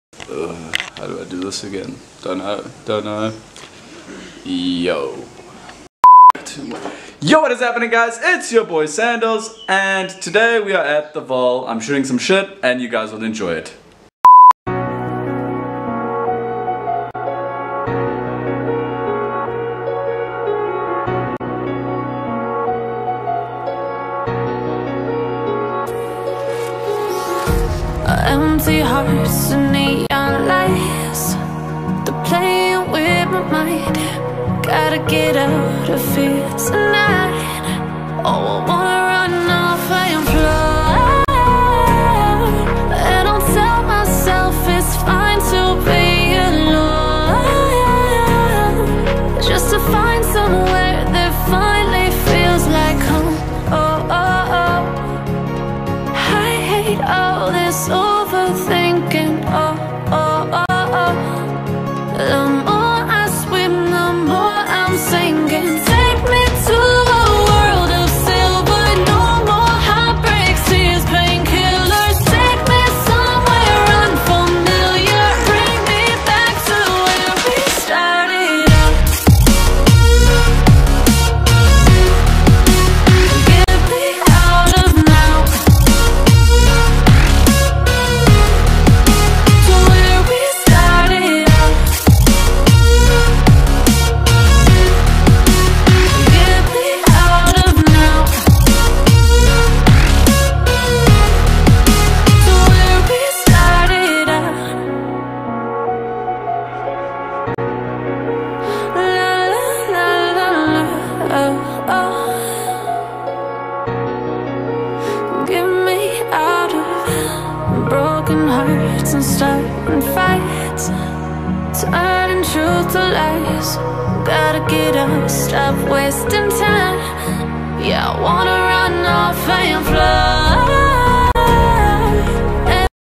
Vacation with The Boys!!
Hi Guys
Here is a Vlog of my friends and i at the Vaal on the weekend, let me know if you like these types of videos by clicking the thumbs up button.